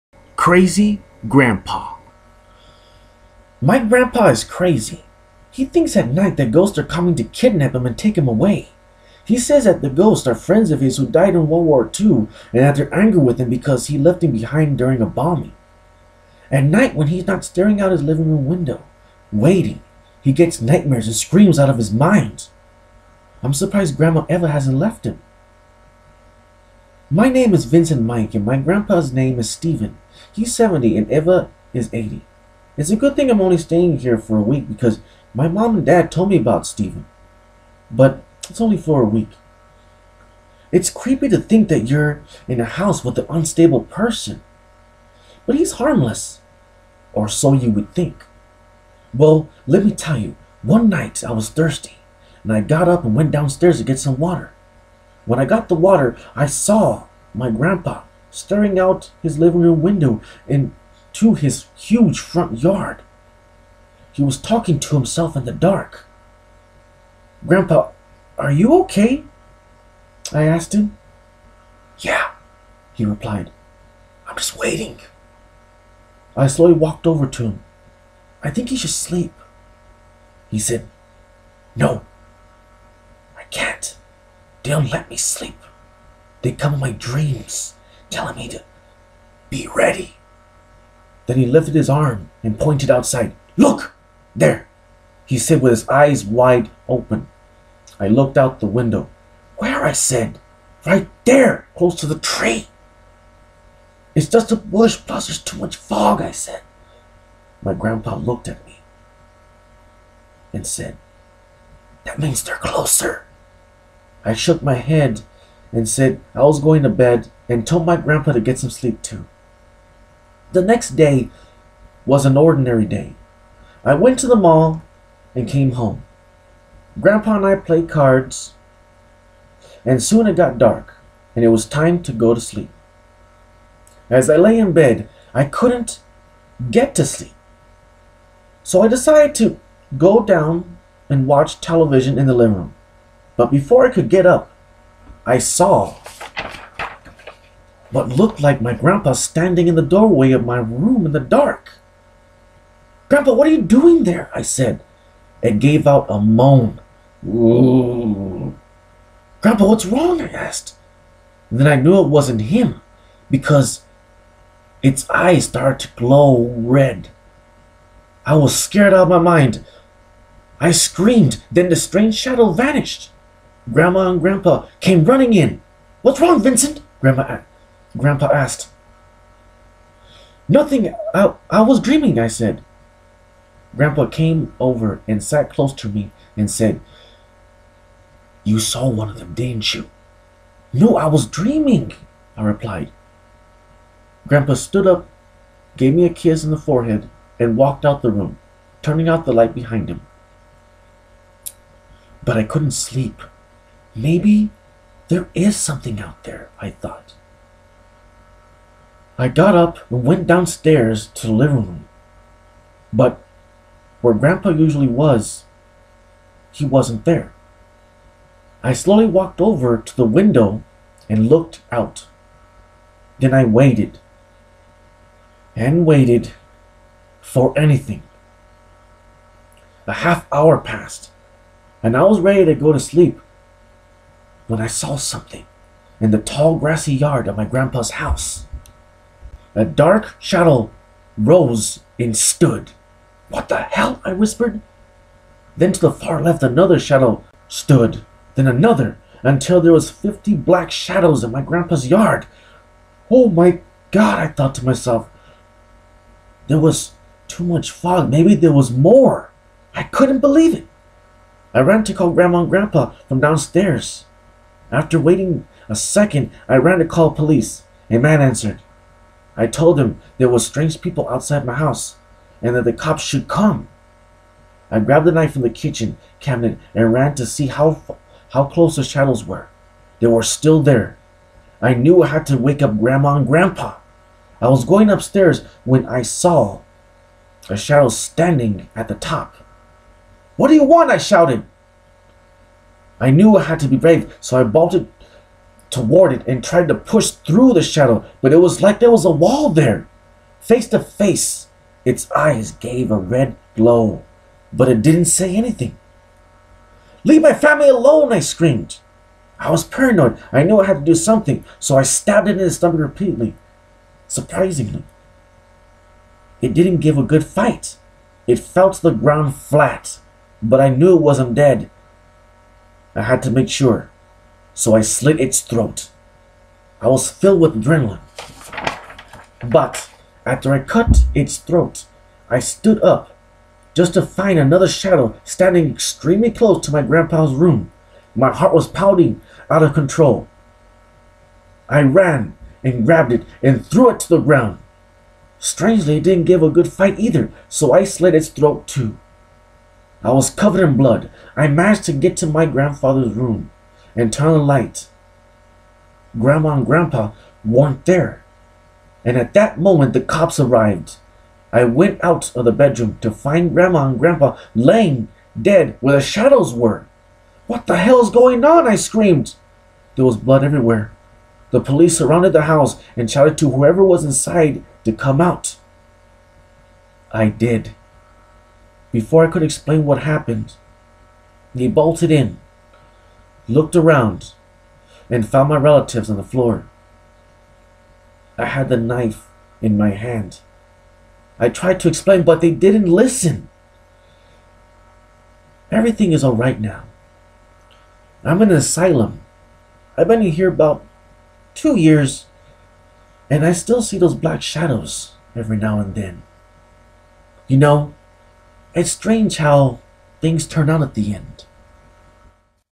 A Scary story "Crazy Grandpa"
Here is a original story I wrote back in 2003 called "Crazy Grandpa". It is inspired by a book I loved as a kid called "Scary stories to tell in the dark". I always wanted to make a good scary story b